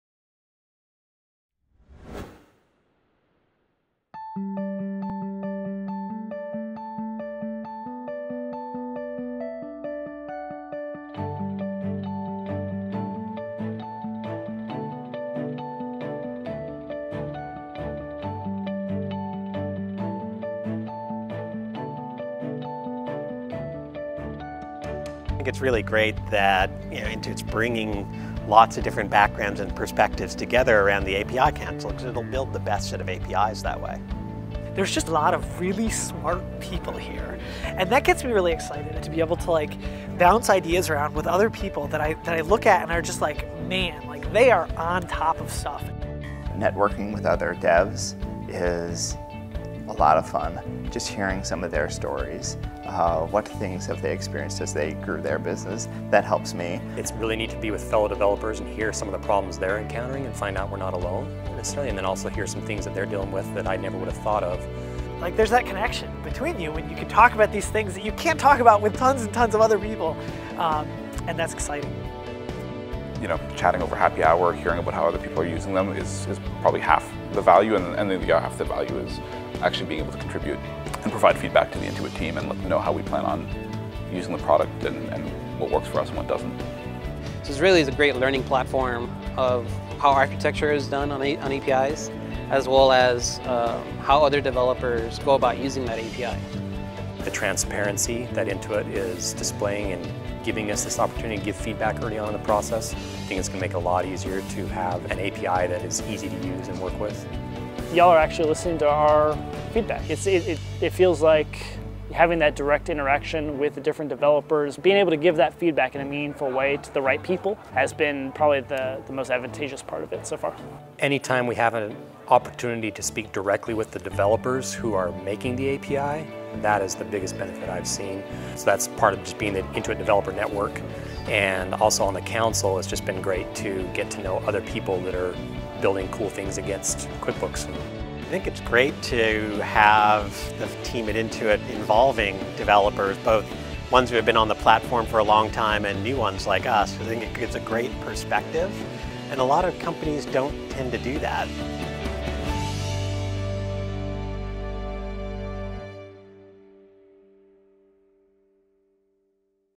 Intuit Developer Council comes to Intuit HQ in Mt View, CA
Hear direct from members of Intuit's Developer Council